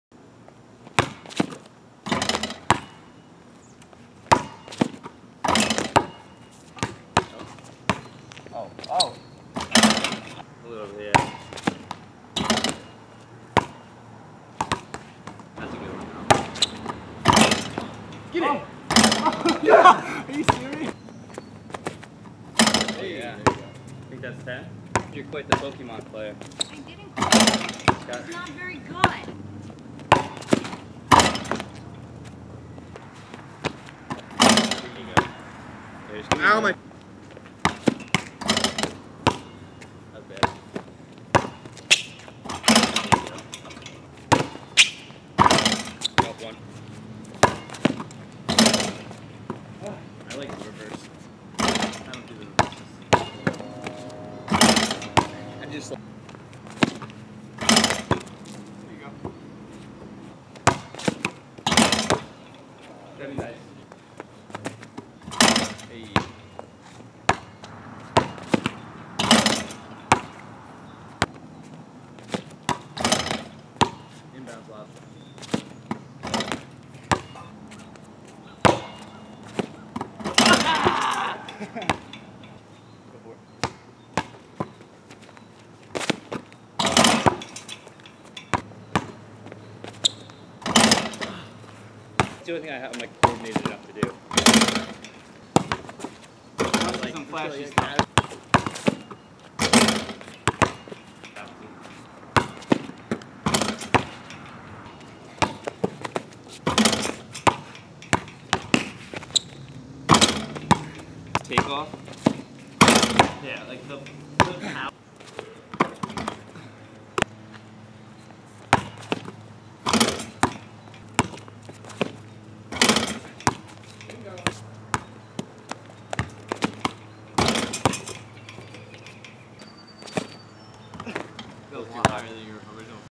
Dunk session w/ Brody & Scott (anotha one)
Dont know what the heights exactly were, just jumping for fun. if i had to guess it was probably about 9'10" Scott is almost 6'5" just giving the height cuz hes never been in a video with me before.